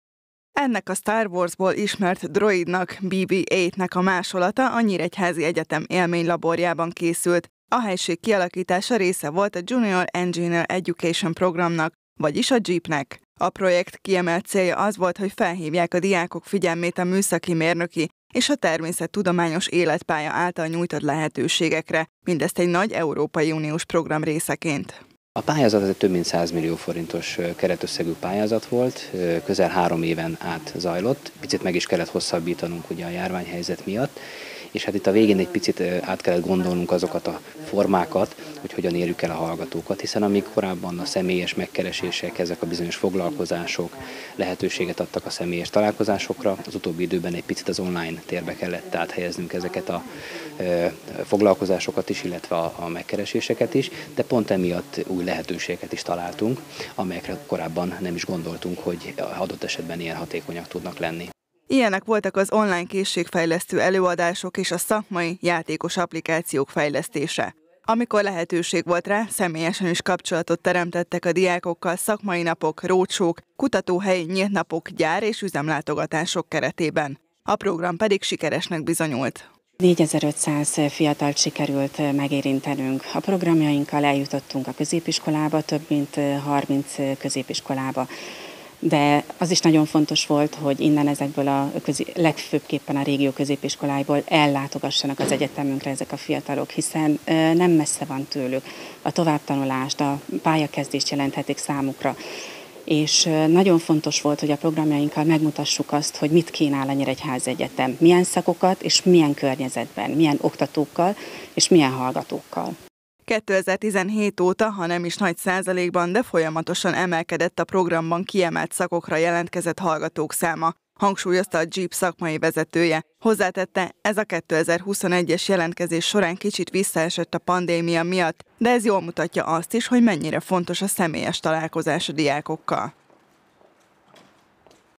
Népszerűek az MTMI képzések - Kölcsey Tv
Több mint négyezer diákot és számos megyei oktatási intézményt ért el a Nyíregyházi Egyetem JEEP című projektjével. Ennek kiemelt célja a MTMI szakok népszerűsítése volt, vagyis hogy ösztönözzék a diákok műszaki, természettudományos, matematika és informatika szakokra való jelentkezését. Ennek keretében számos foglalkozást és gyárlátogatást szerveztek a leendő hallgatóknak.